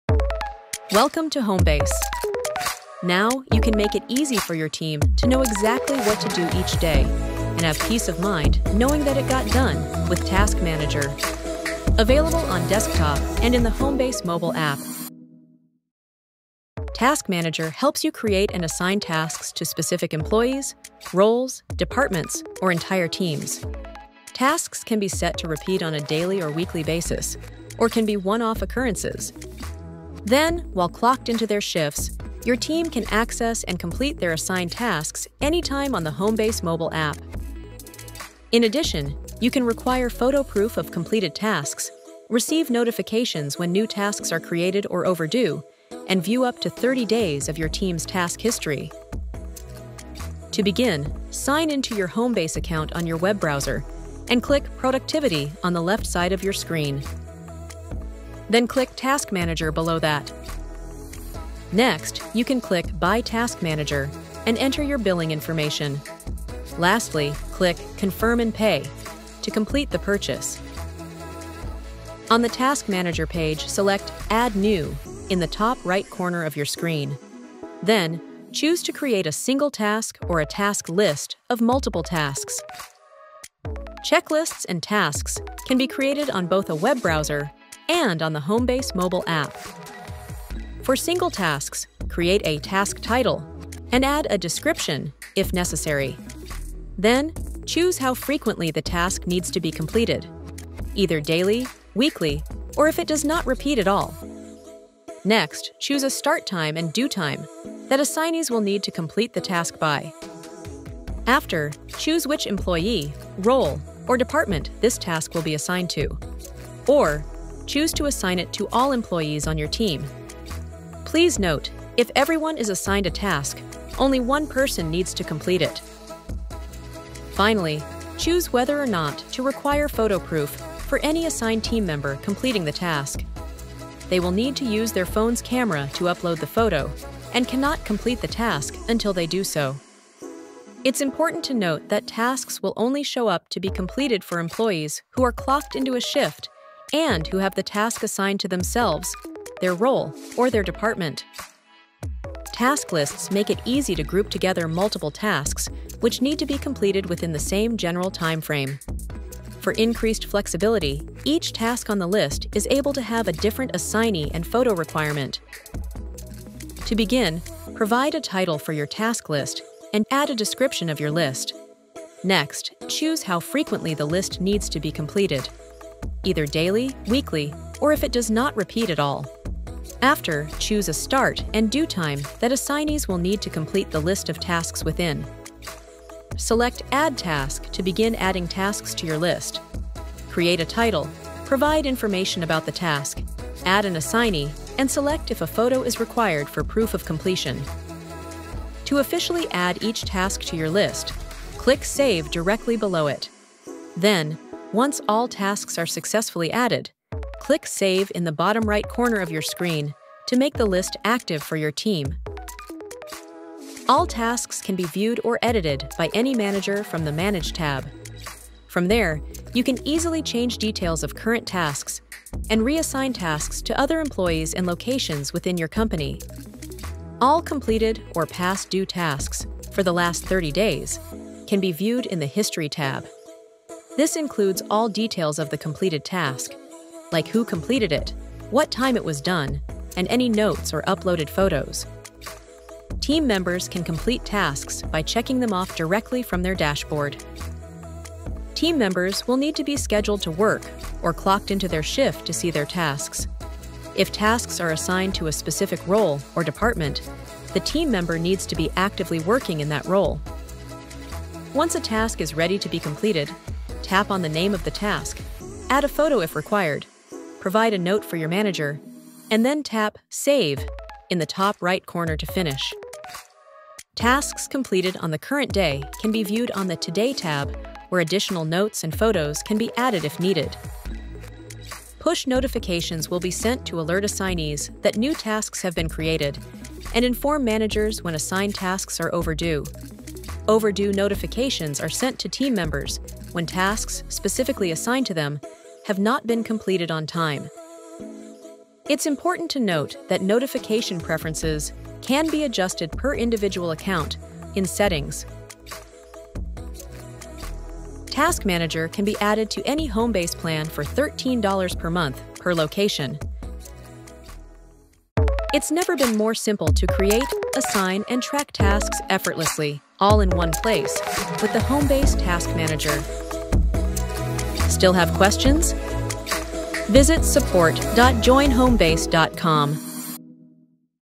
How to Use Task Manager
Make it easier than ever to organize your team’s day and ensure important tasks get completed with the Homebase Task Manager — available on both desktop and the Homebase mobile app!

Task Manager is simple to set up and costs $13/month per location. With Homebase Task Manager, you can create, assign, and track tasks all in one place, giving you and your team greater clarity and accountability.

Skip Ahead:
00:00 Welcome to Homebase
00:16 What is Task Manager?
00:48 Setting Up Task Manager
01:12 Single Task or Task List
01:30 Creating Single Tasks
02:21 Making Sure Tasks Show Up for Team Members
02:31 Creating a Task List
03:47 Task History
04:02 How to Complete Tasks
04:41 New Task & Overdue Notifications
05:24 More Questions?